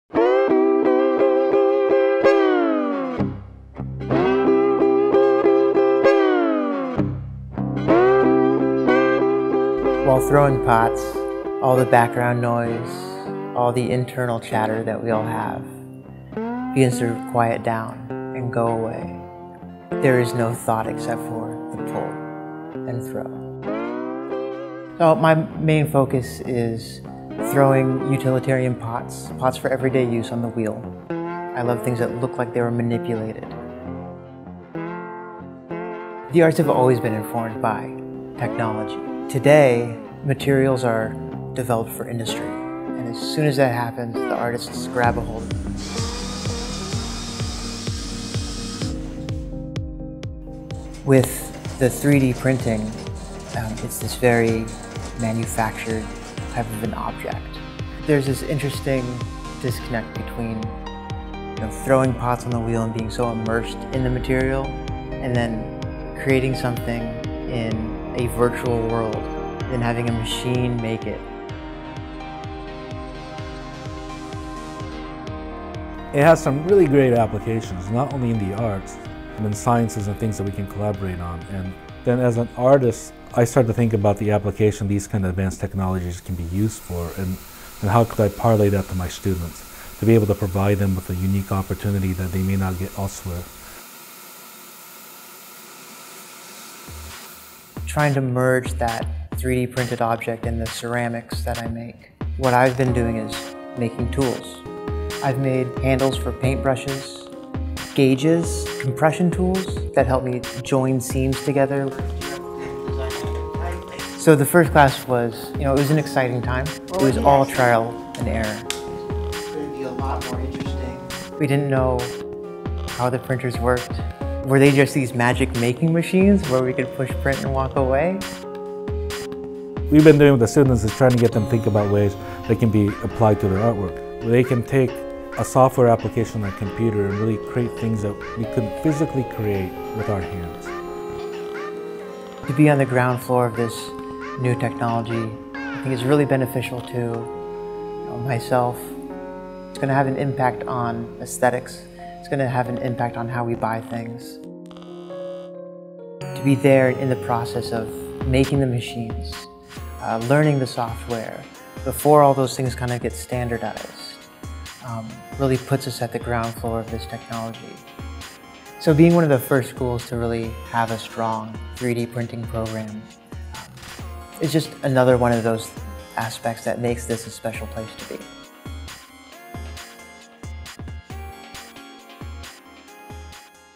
Art in 3D: Ceramic 3D Printing in the WVU College of Creative Arts
Imagination is your guide in the ceramic 3D printing program at West Virginia University. The WVU College of Creative Arts is an early pioneer in blending 3D printing with art in a college setting and is one of only a few programs offering this high-tech art experience nationwide.